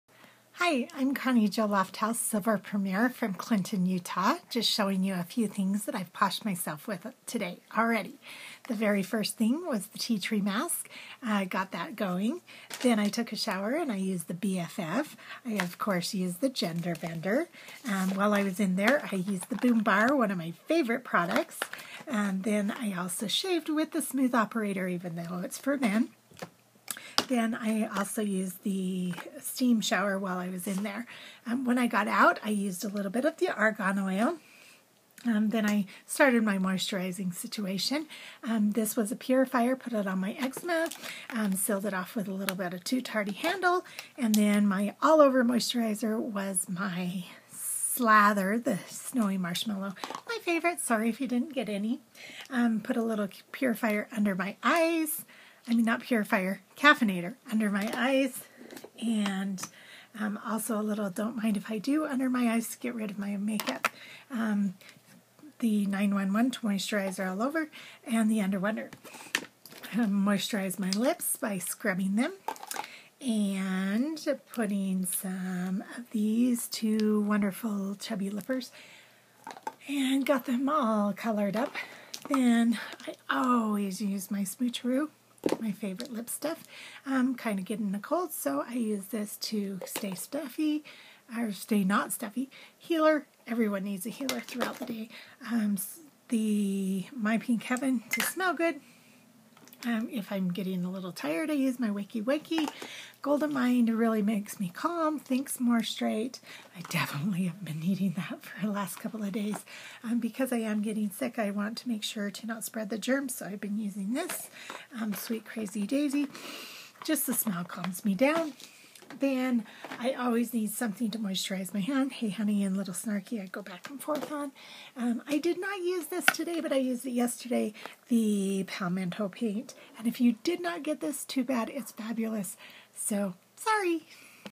Look At All the Perfectly Posh Products I've Used Today
All the Perfectly Posh pampering products I used today, because I Deserve It! It's been a rough couple of days, we all need some pampering when we are experiencing stress...Who am I kidding, we all need pampering ALL THE TIME. We don't need an excuse. Find an excuse!